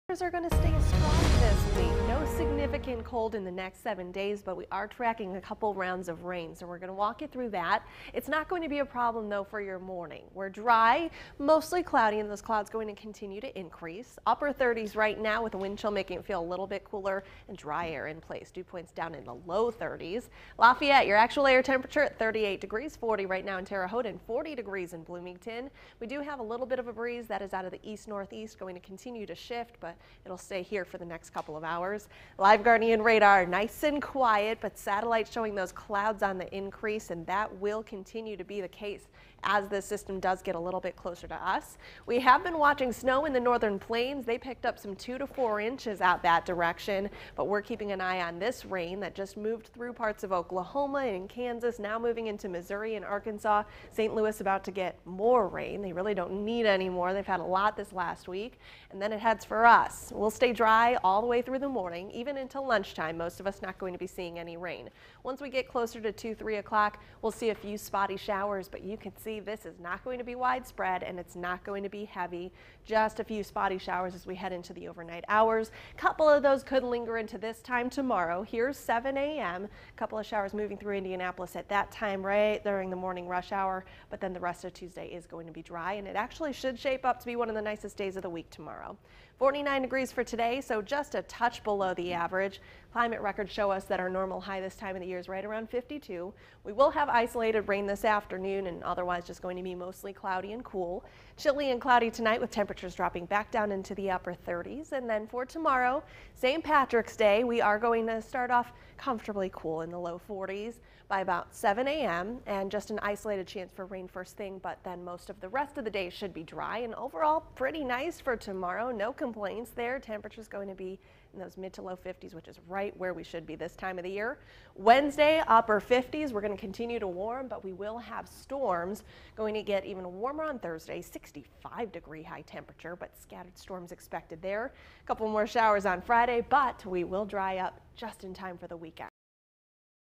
Spotty showers this afternoon; warmer tomorrow
Meteorologist Star Derry has your central Indiana weather for this Monday, March 16 and the week ahead.